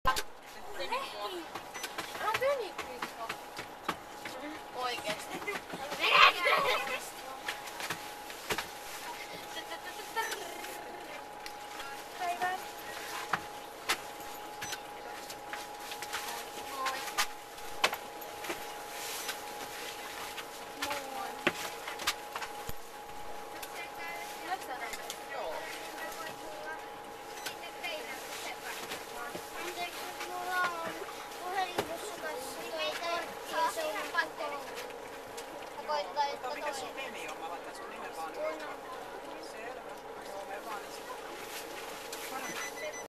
CHILDREN TRAVEL WINTER SCHOOL BUSS HOME FOR iPHONE12 PRO4K 5GNETS SAMSUNG G S20 ULTRA 5G &5GANDROID
ON COLD WINTER DAYS SCHOOL CHILDREN WEAR WARM
OVERCOATS,SCARFS,HATS WHILE WAITING FOR THE
SCHOOL BUSS TO TAKE THEM HOME & QUICKLY STAND
IN LINE TO BOARD THE BUSS.SEND SHARE WINTER SCHOOL
MEMORIES OF SCHOOL BUSS TRIPS & PLAYGROUND FUN
SEARCH FOR MORE IGSP SHARES HIS ADVENTURES
YOU TUBE FINLAND